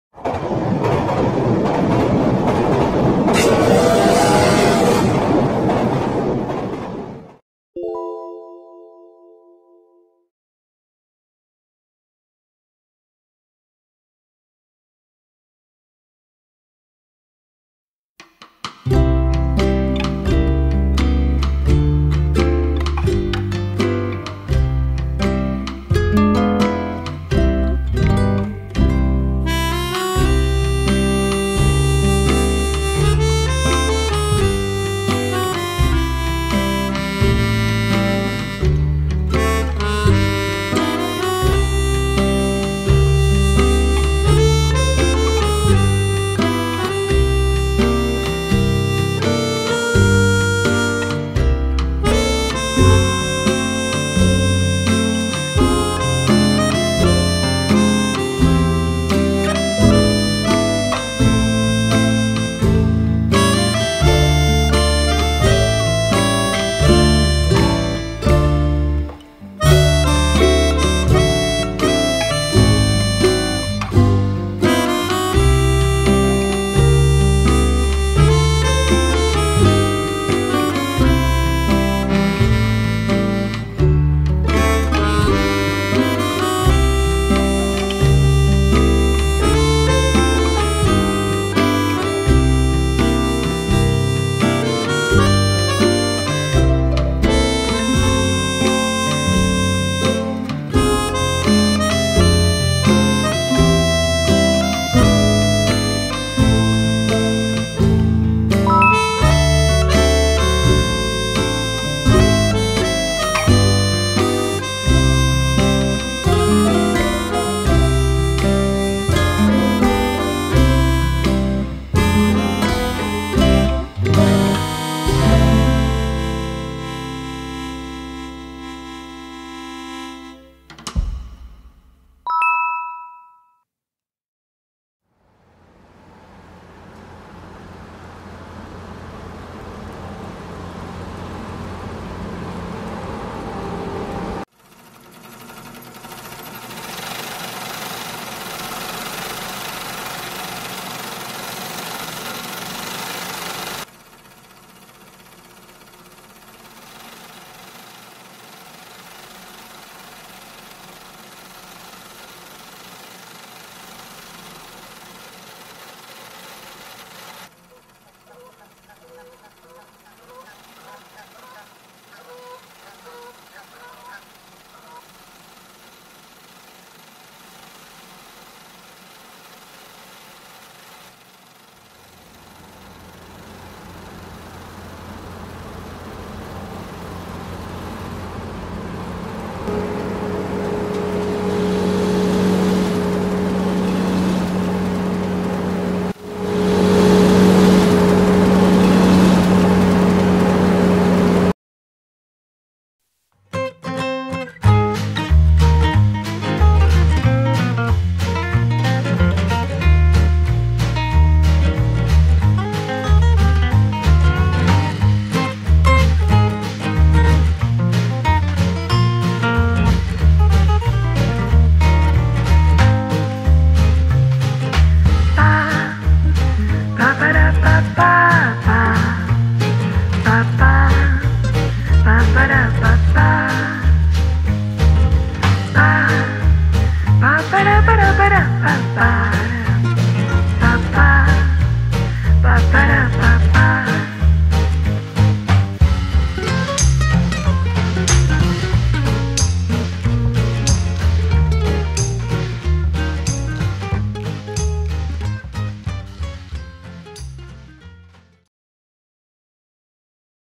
緑茶が湧いた？ The dig site Nの時間 vol.159
暑い暑い夏の午後
喉の渇きのせいか
砂漠の掘削現場を思いついた

【音楽提供】音楽研究所
【音楽提供】甘茶の音楽工房
【音楽・効果音提供】ムズムズ音楽素材
【音楽・効果音提供】エスフェクツ効果音素材配信
  Sound provided by Esffects